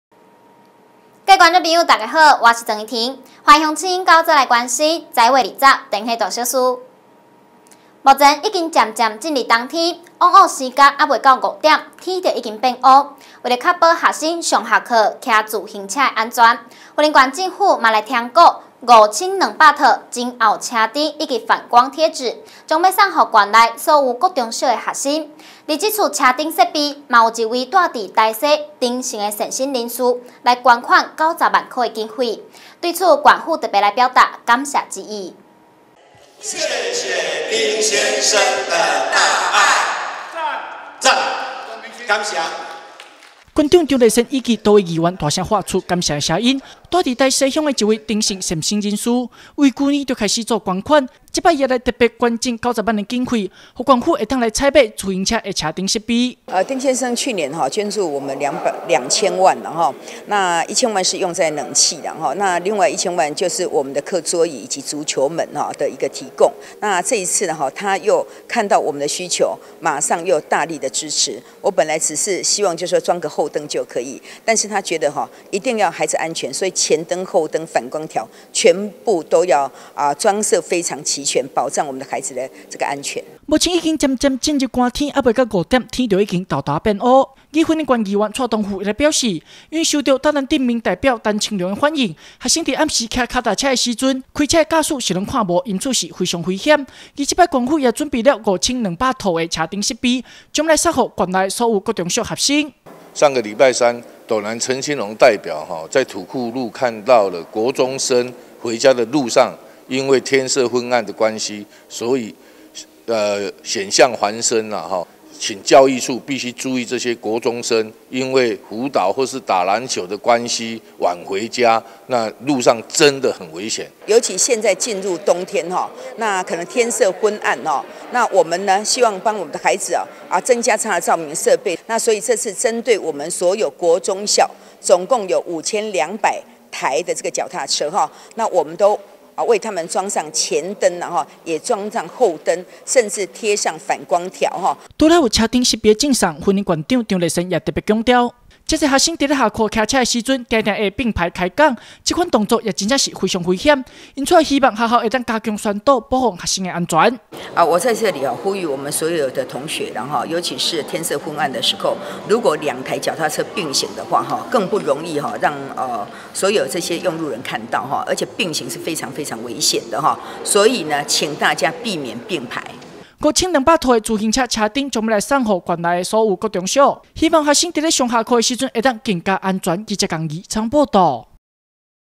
雲林新聞網-斗六學生自行車車燈安裝
雲林新聞網-縣府贈5200套車燈 助學生安心返家
目前已經漸漸進入冬天，往往時間還沒到五點天就已經變暗，為了確保學生上下課騎自行車的安全，雲林縣政府也來添購5200套的，前後車燈以及反光貼紙，將要送給縣內的所有的國中小學生，而針對這次的車燈設備，也有位住在台西的丁姓善心人士，來捐款九十萬元，對此縣府也特別來表達感謝之意。